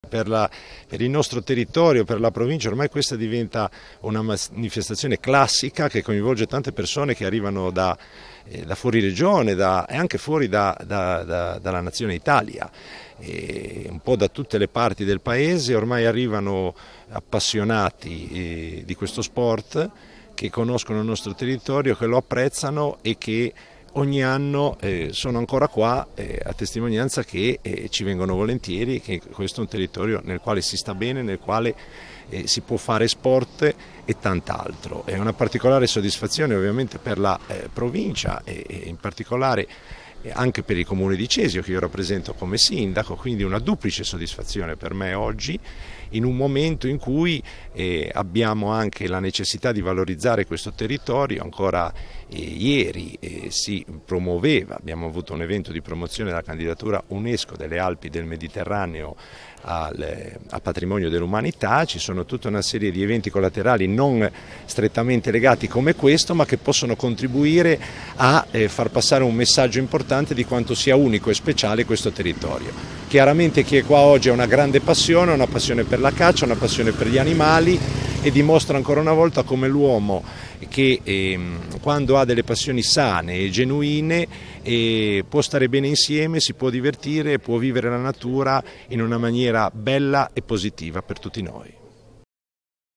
FABIO NATTA AL RADUNO CINOFILO DI COLLE SAN BARTOLOMEO
Parliamo ancora del Raduno dei cinofili del Colle San Bartolomeo. Era infatti presente anche il presidente della provincia di Imperia Fabio Natta, che ha espresso soddisfazione per il grande richiamo che ha ottenuto l’evento.